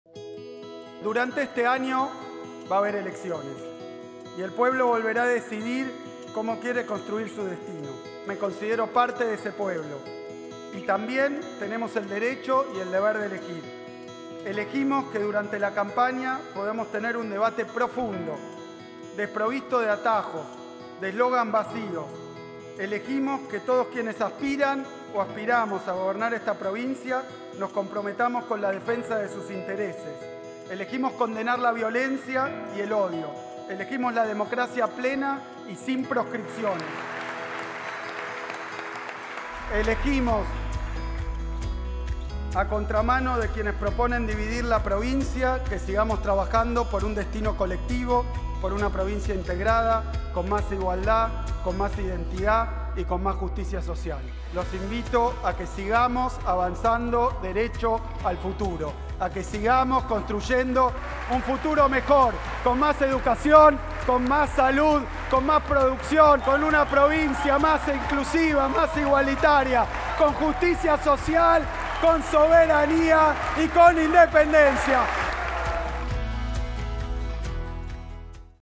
Destino colectivo | Sesión especial ante la Legislatura Bonaerense
Este año habrá elecciones y el pueblo volverá a decidir cómo quiere construir su destino.

Las y los invito a que sigamos trabajando por un destino colectivo, por una provincia integrada, con más igualdad, con más identidad y con más justicia social.

Sigamos avanzando